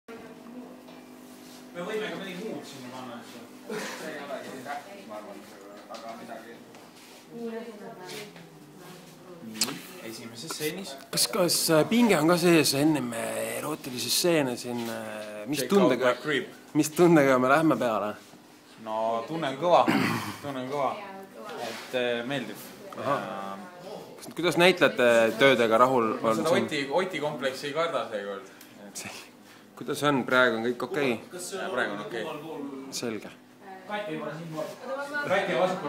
Nurjatud tüdrukud behind the scenes
Kõpper selgitab väikse Oti seisu.